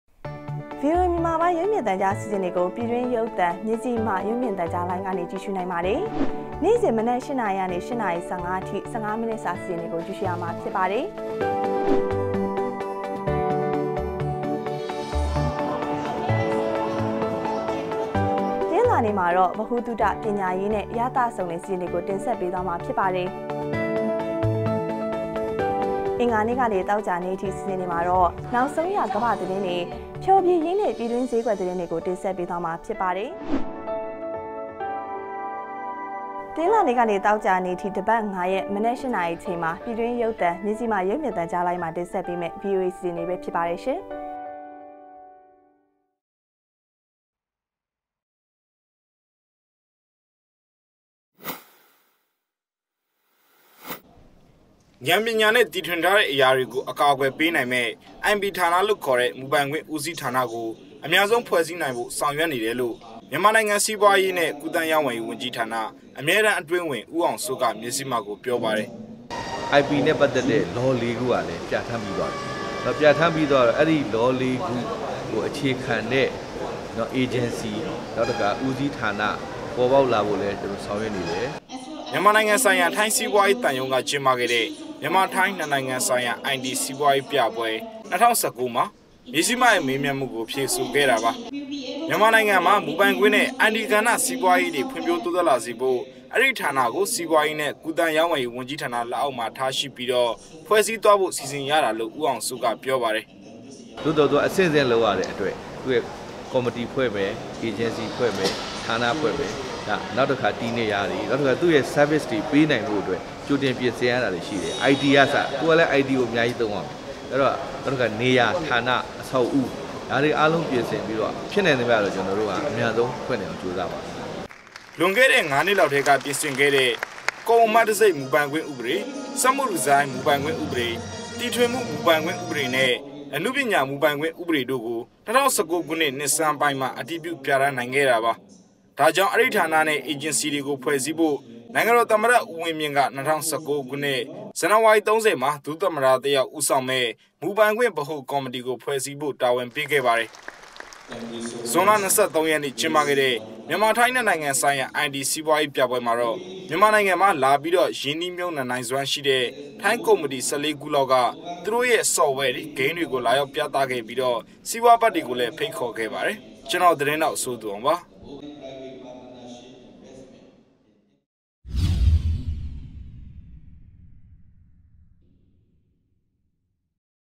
မူပိုင္ခြင့္ဦးစီးဌာနကိုအျမန္ဆံုးဖြဲ႕စည္းေရးေဆာင္ရြက္ေနဟု စီးပြားကူးသန္းအၿမဲတမ္းအတြင္းဝန္ေျပာ
ဥာဏ္ပညာျဖင့္တီထြင္ဖန္တီးထားေသာအရာမ်ားကို အကာအကြယ္ေပးႏိုင္မည့္ IP (Intellectual Property) ဌာန (ေခၚ) မူပိုင္ခြင့္ဦးစီးဌာနကို အျမန္ ဆံုးဖြဲ႕စည္းႏိုင္ေရးအတြက္ ေဆာင္ရြက္ေနေၾကာင္း ျမန္မာႏိုင္ငံ စီးပြားေရးႏွင့္ ကူးသန္းေရာင္းဝယ္ေရးဝန္ႀကီးဌာနမွ အၿမဲတမ္းအတြင္းဝန္ ဦးေအာင္စိုးက မဇိၥ်မ သို႔ေျပာသည္။

“IP နဲ႔ပတ္သက္တဲ႔ ဥပေဒ (၄) ခုကလည္း ျပဌာန္းၿပီးသြားၿပီ။ အဲ႔ဒီ ဥပေဒ (၄) ခုကိုအေျခခံတဲ႔ ေအဂ်င္စီ၊ ေနာက္တစ္ခါ ဦးစီးဌာနအေပၚ ေပါက္လာဖို႔လည္းေဆာင္ရြက္ေနတယ္” ဟု ၎ကဆိုသည္။

ျမန္မာႏိုင္ငံဆိုင္ရာ ထိုင္းစီးပြားေရးသံရံုးမွက်င္းပခဲ႔သည့္ “ ျမန္မာ-ထိုင္းႏွစ္ႏိုင္ငံဆိုင္ရာ အိုင္တီစီးပြားေရးျပပြဲ ၂၀၁၉” ပြဲတြင္ မဇိၥ်မ၏ေမးျမန္းမႈ ကိုေျဖဆိုခဲ႔ျခင္းျဖစ္သည္။

ရိုက္ကူး / တည္းျဖတ္ -  စိုးသူေအာင္